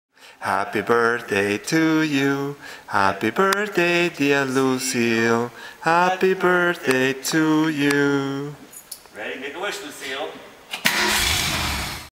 It's Fabian's 63 Falcon, Lucille's, Birthday-KmartDesign
Kmart designer, Fabian, has a birthday announcement.  Luille, his 63 Falcon, is another year older and still looking fabulous.